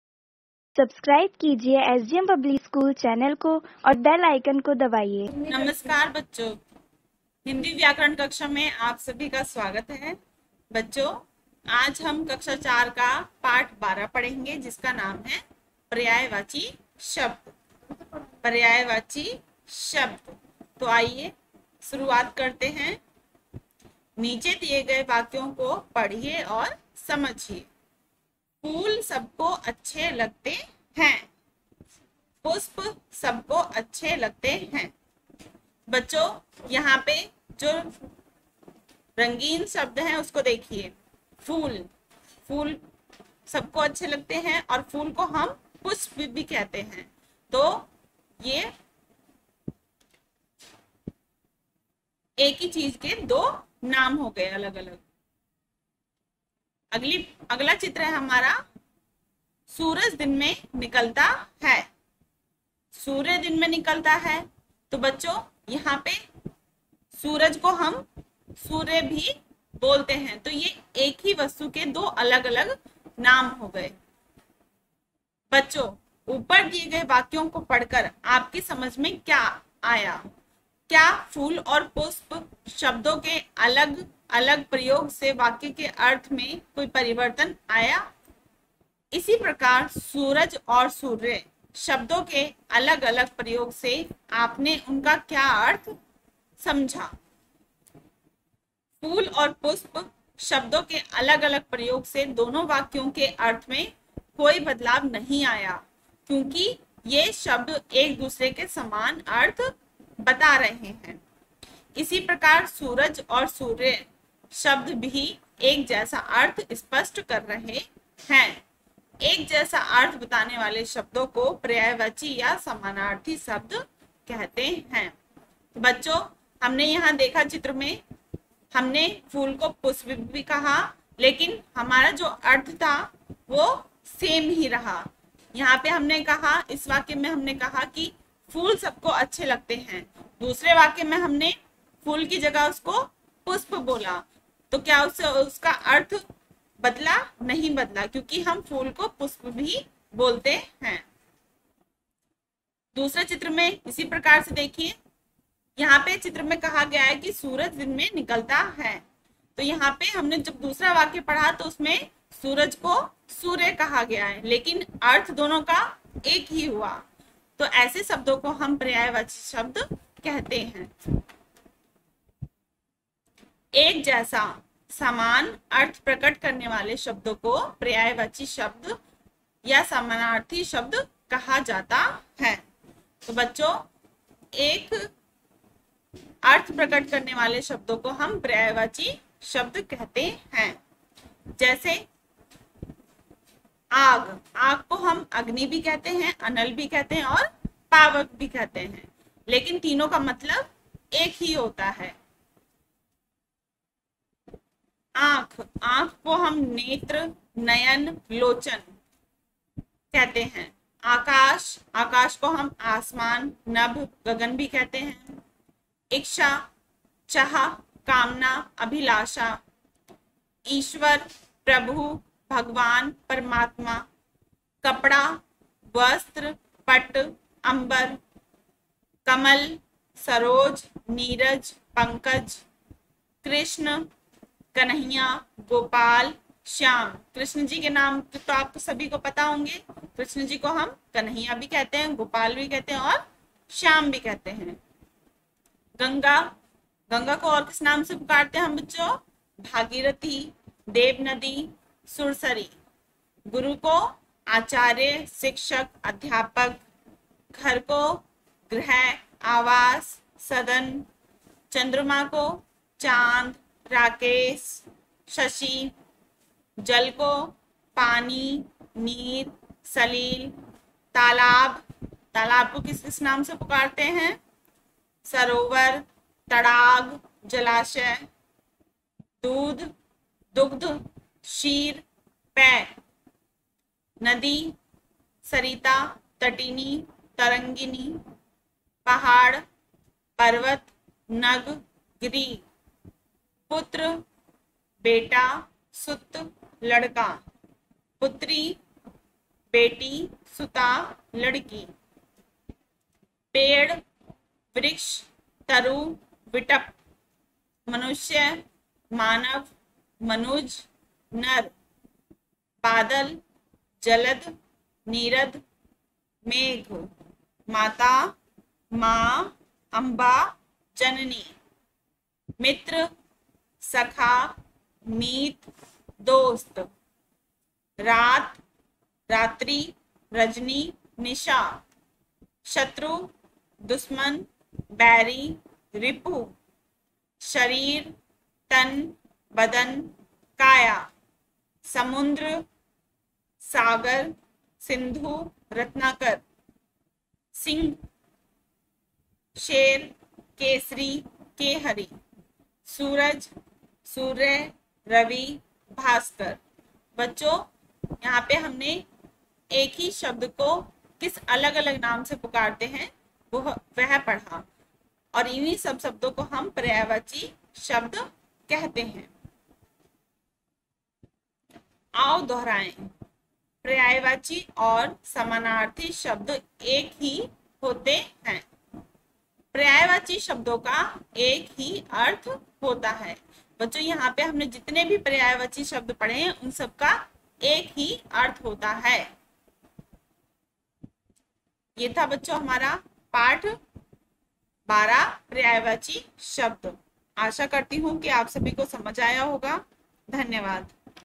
Class 4th | Hindi ( G ) | Chapter - 12 | S.D.M. PUBLIC SCHOOL | 2021-2022
बच्चो आज हम कक्षा चौथी हिंदी व्याकरण की पाठ्य पुस्तक  का पाठ 12 पढ़ेगे | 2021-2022
  by S.D.M. Public School Delhi-84